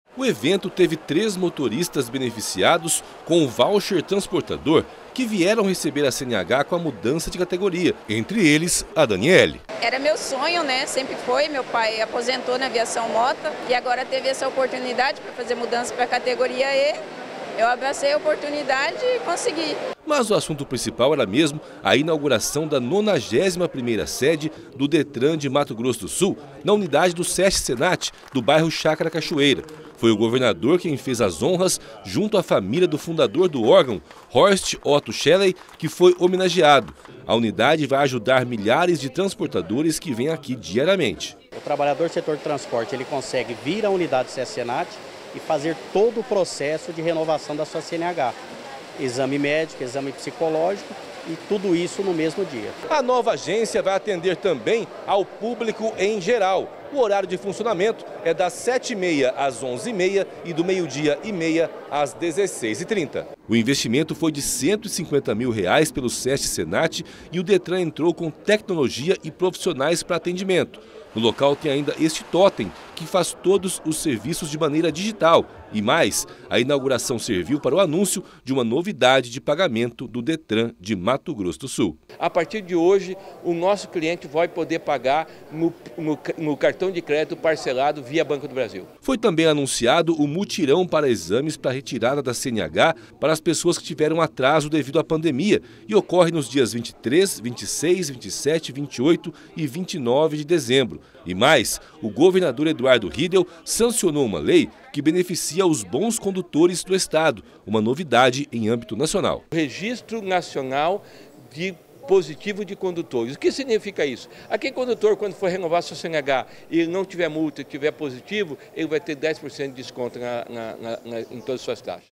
Governo sanciona lei que beneficia os bons condutores de Mato Grosso do Sul com descontos em taxas
Governo do Estado sancionou a lei que beneficia os bons condutores com descontos em taxas. O anúncio foi feito durante a inauguração da 91ª agência do Detran em Mato Grosso do Sul.